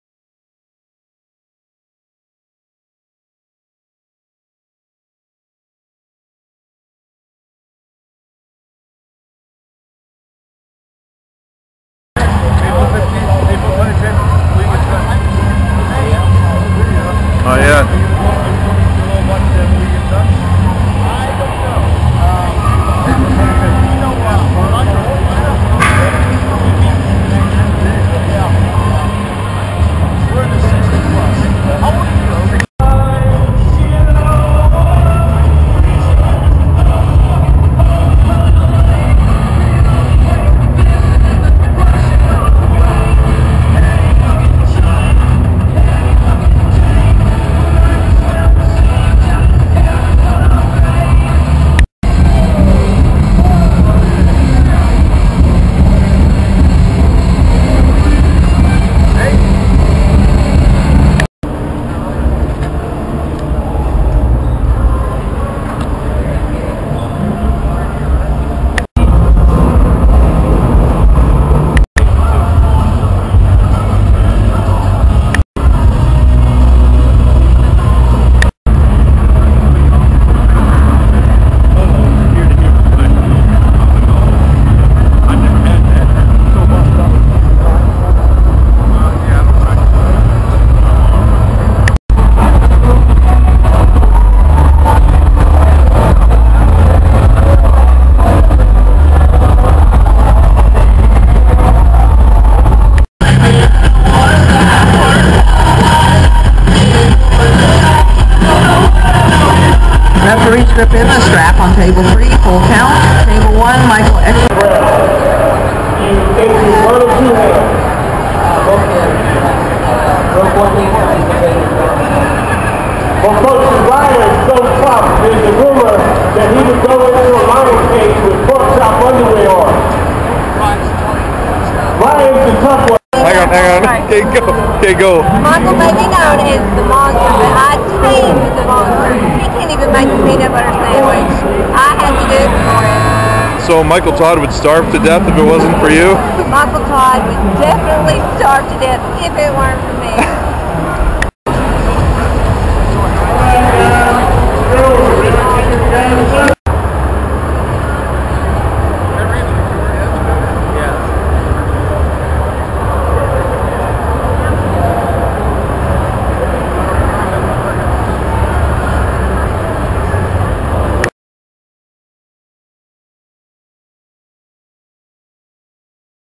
RyanEspey.com -- March 5, 2010 -- Around the Arnold's - Part 2
March 5, 2010 -- Around the Arnold's - Part 2

Around the Arnold Expo on day 1 of the actual event. There is some armwrestling, strongman, and a few other random things. Watch for Tonya Todd's comments about her husband the "Monster"... it is great stuff.